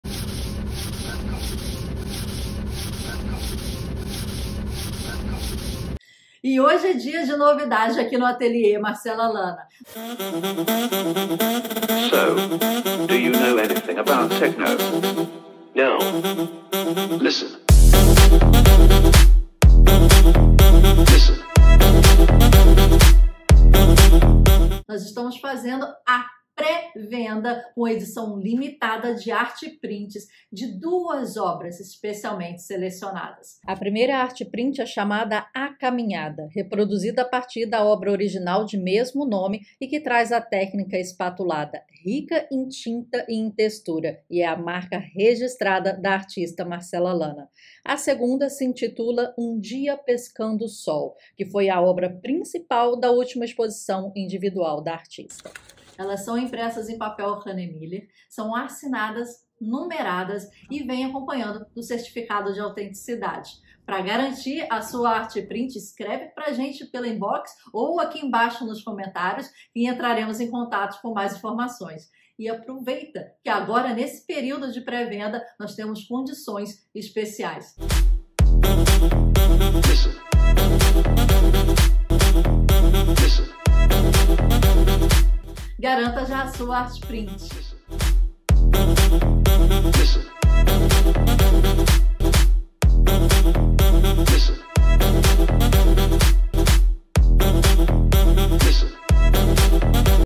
Art Prints da artista Marcela Lanna - Lançamento | MaHub
*PRÉ-VENDA DE ART PRINTS*

Temos novidades no Atelier Marcela Lanna. Estamos fazendo a PRÉ-VENDA com edição limitada de ART PRINTS de duas (2) obras especialmente selecionadas.

As Art Prints são impressas em papel Hahnemühle, assinadas pela artista, numeradas e acompanham o certificado de autenticidade em português/inglês.

Aproveite!! Temos condições especiais para nossa Pré-Venda!

ART PRINT #1
Título: A Caminhada
Medida: 60x44cm (23,6x17,3in)

ART PRINT #2
Título: Um Dia Pescando Luz
Medida: 42x60cm (16,5x23,6in)

JÁ CURTA GRATUITAMENTE NOSSO CLUBE DO LIVRO MaBookClub:

🟣 Amazon (Página Pessoal com opiniões sobre livros)